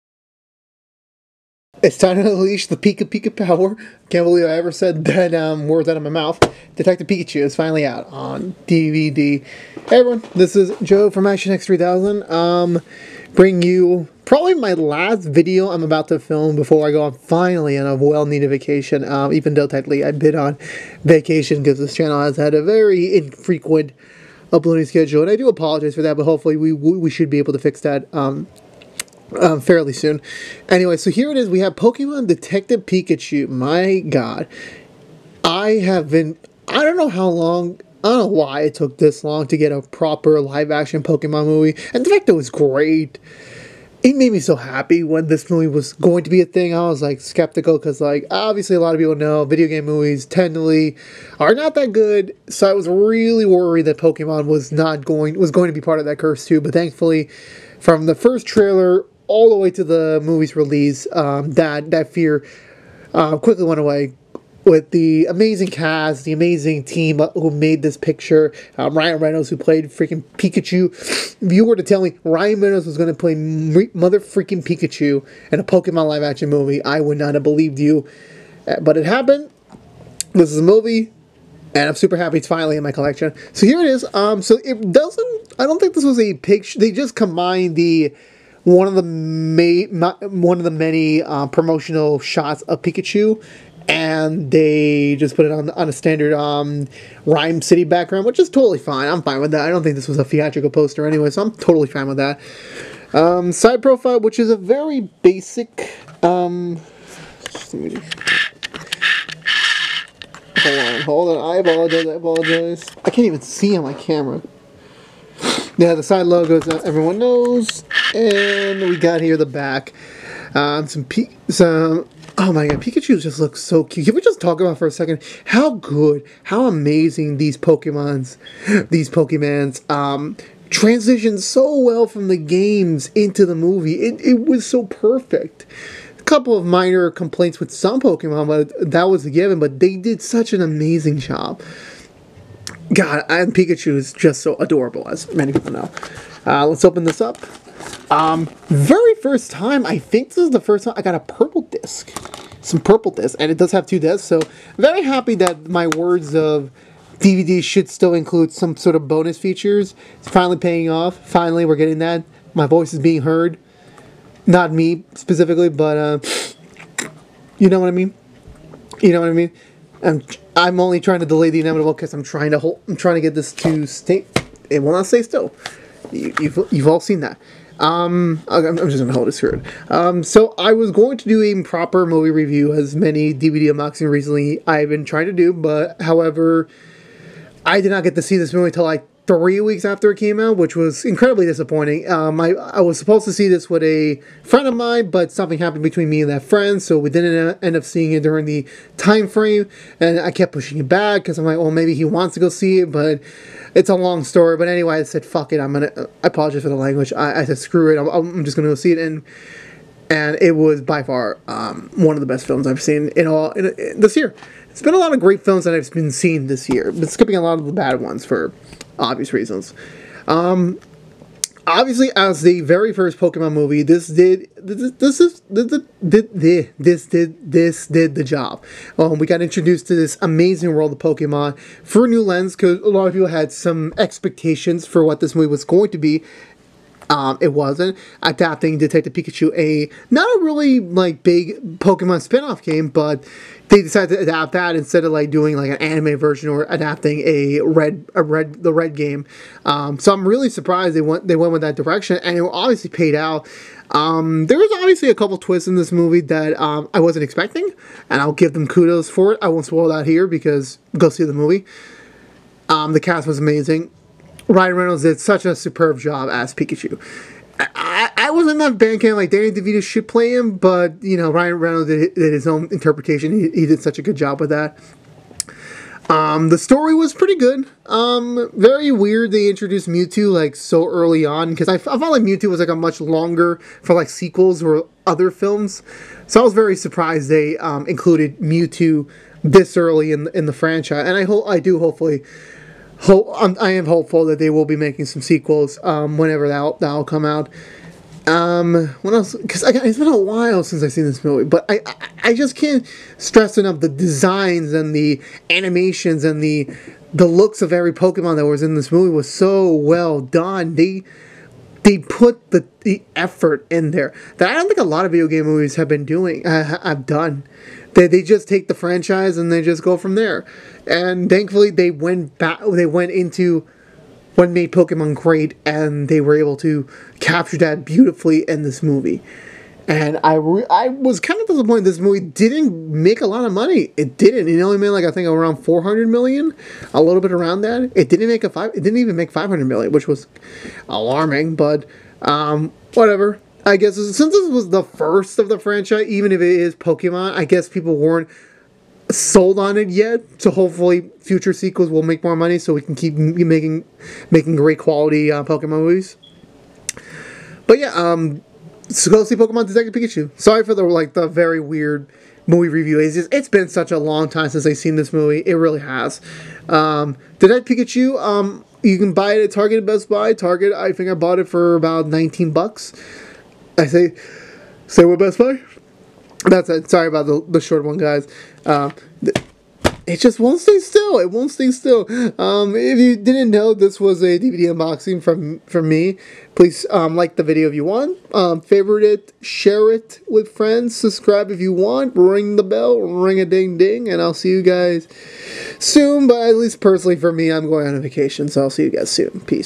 Detective Pikachu - DVD Unboxing/Review
Pokemon finally got the big screen treatment earlier this year and now has been released on DVD, so I better take a look at it.